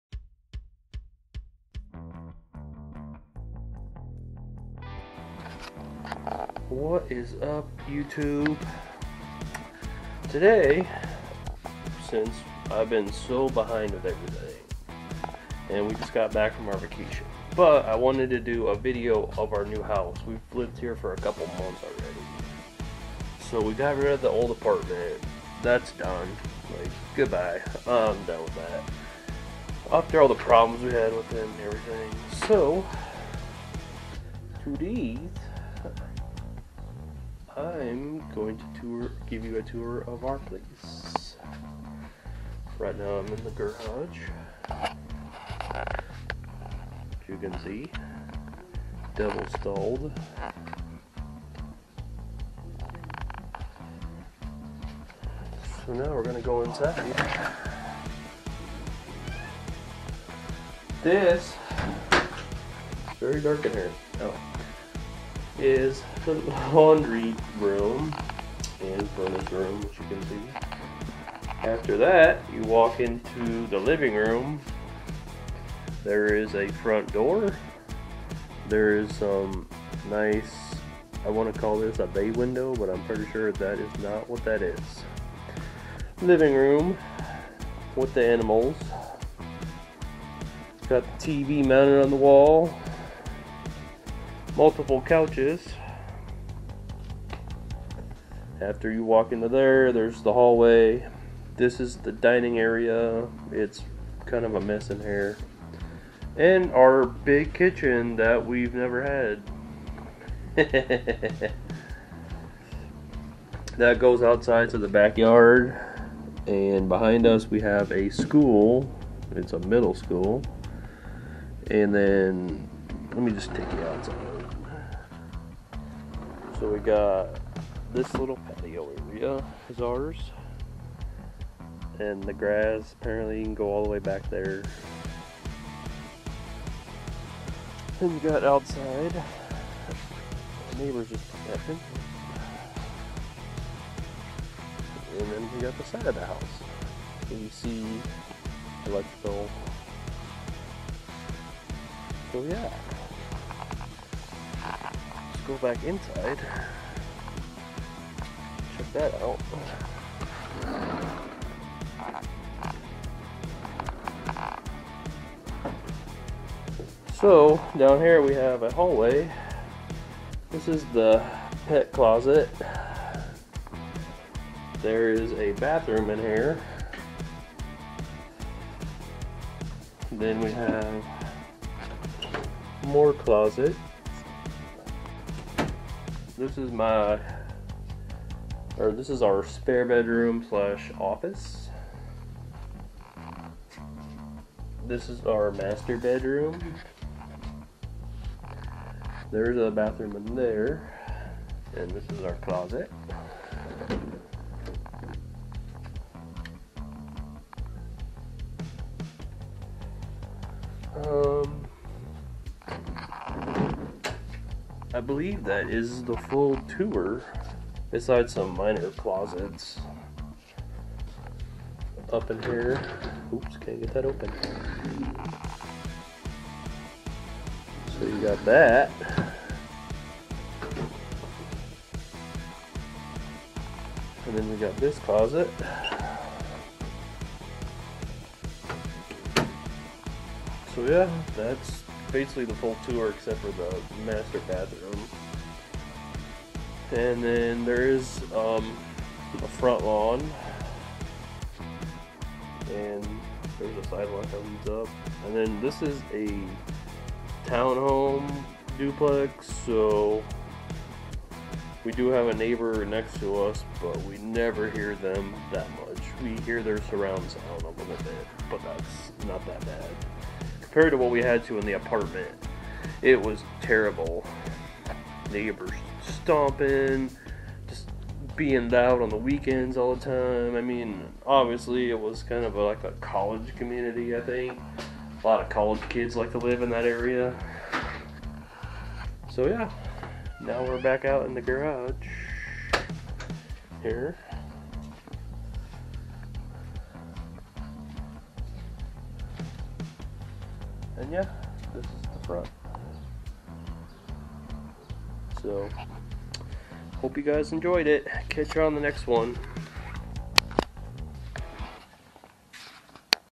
We Moved!!.. well 2 months ago
I have been wanting to make a video of the house we moved into, been slacking on getting footage of it.
so i hope you enjoy the tour of a house.
not huge but its way better then the apartments we lived in and much more quiet here.